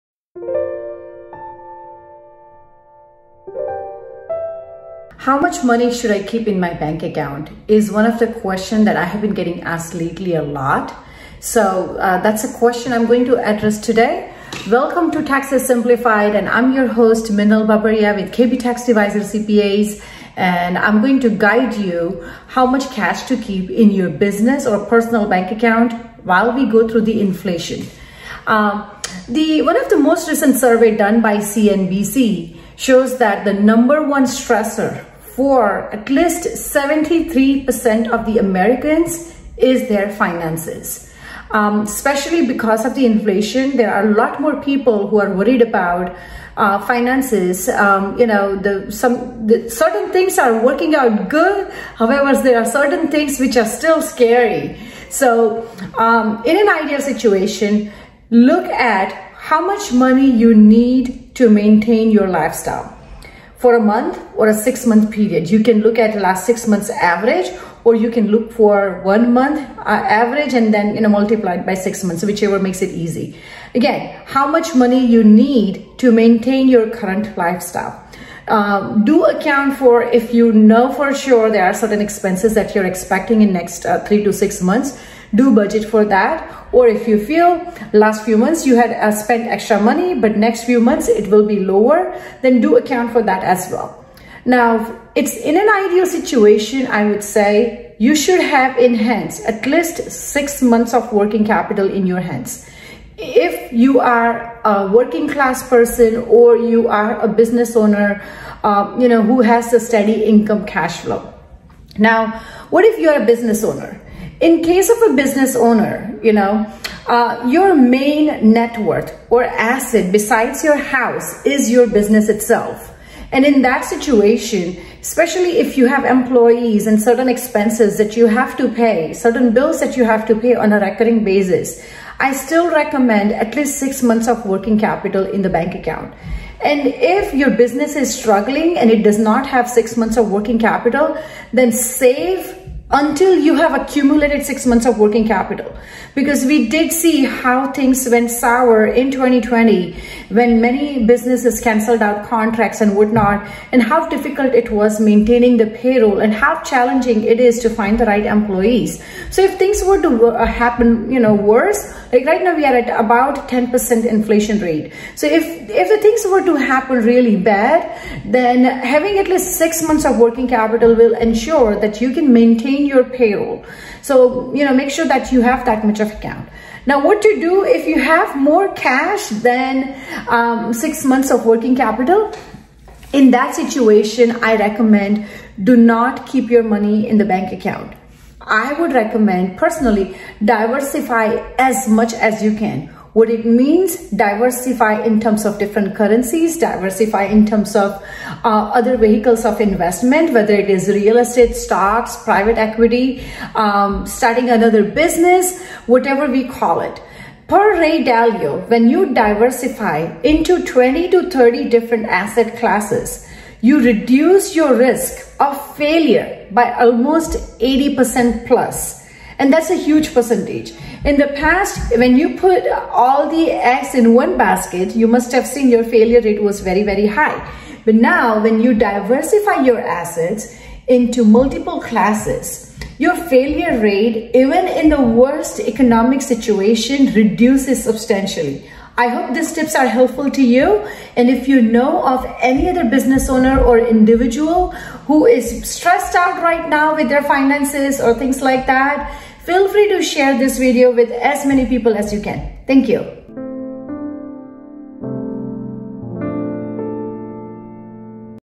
Taxes Simplified: How Much Liquid Cash Should I Keep In My Bank Account?
In this time of inflation, how much liquid cash should you keep in your bank account?

Watch this video and check out what monetary decision fits in this crucial time.

Are you ready to change the way you handle your finances?